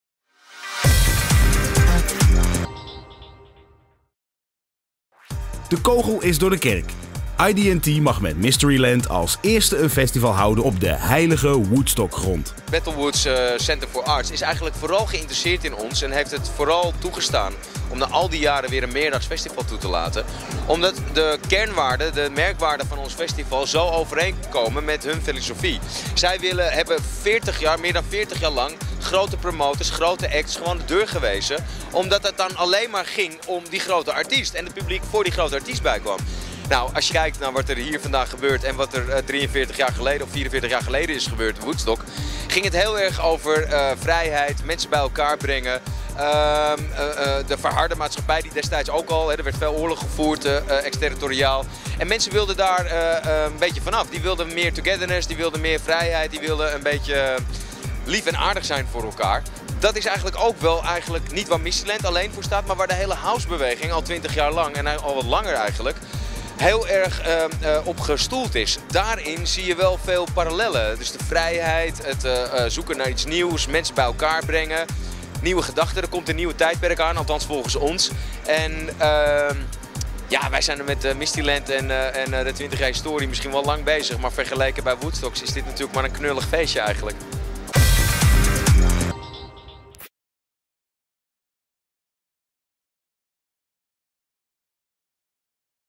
ID&T is to organize Mysteryland on holy Woodstock grounds (EDN Headlines)
Dutch party promoter ID&T has granted permission to organize their Mysteryland festival at the holy festival terrain of Woodstock '69. Bethel Woods Centre for the Arts has rejected others for 44 years.